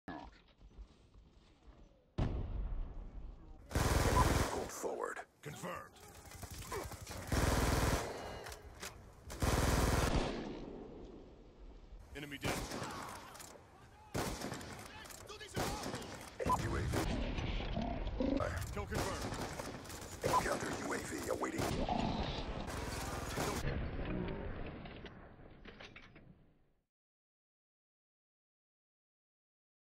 Macau: The Killchain
Pretty cool Killchain I got on Livestream :D Almost got the 5 on screen at the end :(